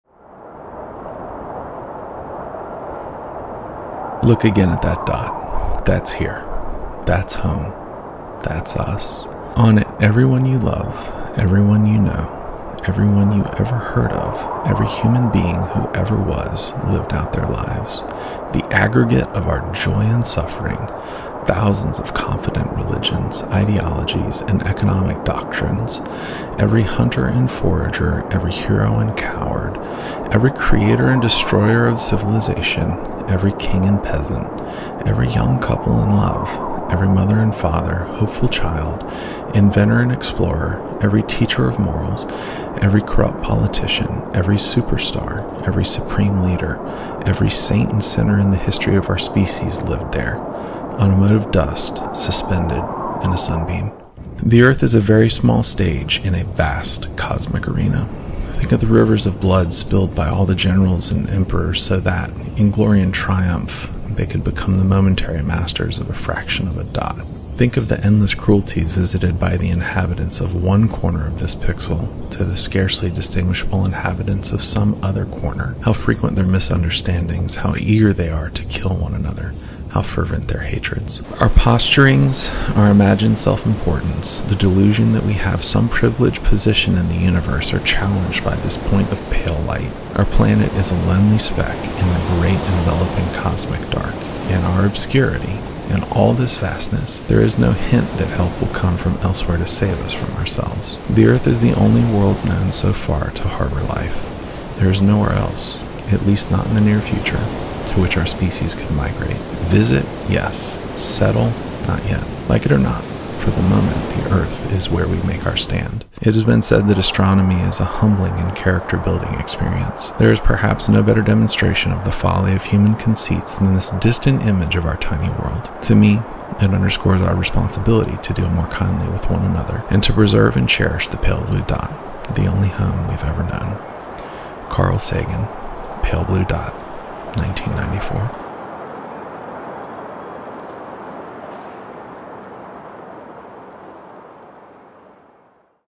Pale Blue Dot - FPV
This is something experimental for me.  Folks seem to like some of the voice over work I've done so I went a new direction with a flight edit.  Obviously, this isn't my footage, but this speech deserves the best footage to be found, and the gentlemen above provided it.